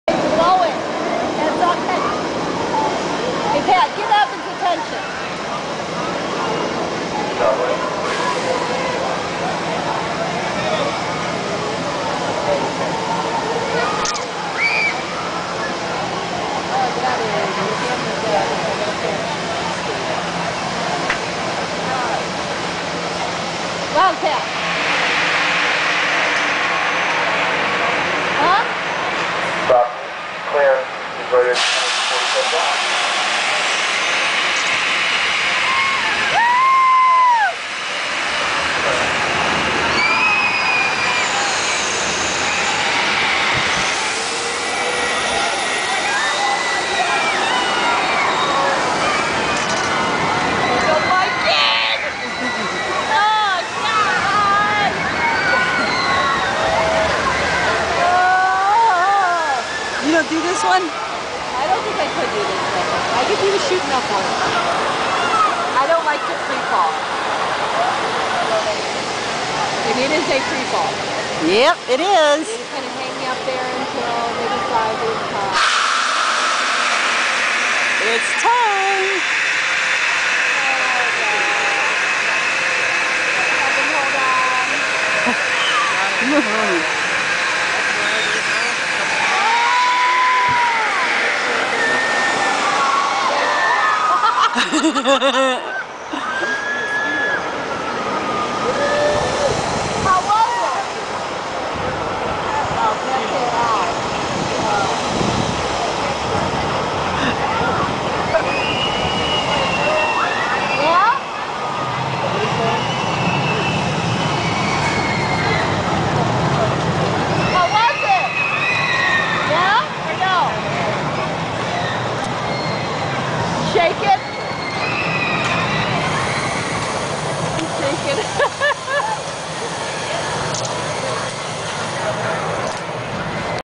Cedar Point Power Tower
Evan rides Power Tower's Blue side - the one that DROPS you 240' - for the first time ever w/his Dad, Patrick on Cedar Point's opening day.